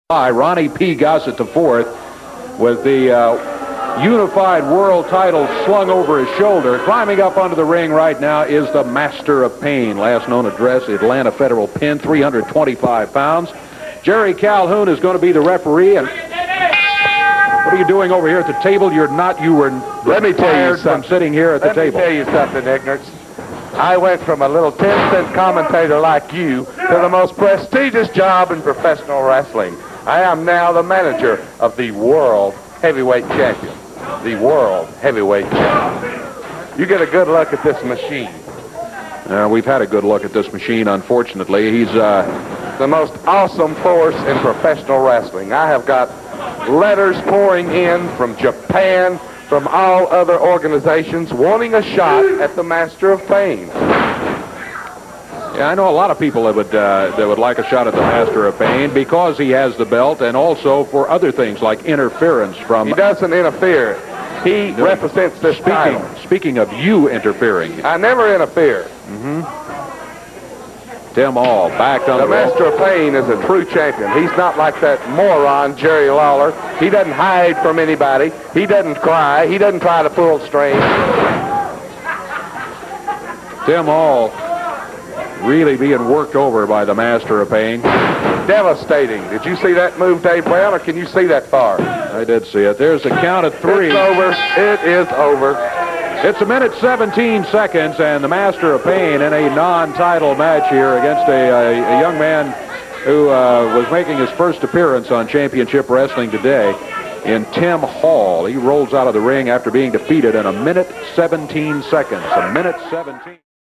اندرتيكر '' في الثمانينات تحديدآ في عام [ 1989 ] قبل تجسيده لشخصية [ الأندرتيكر ]
فيديو يظهر فيه اندرتيكر قبل تجسيده شخصية الأندرتيكر ، هذا الفيديو تم تصويره في عام 1989 ، فيديو نادر.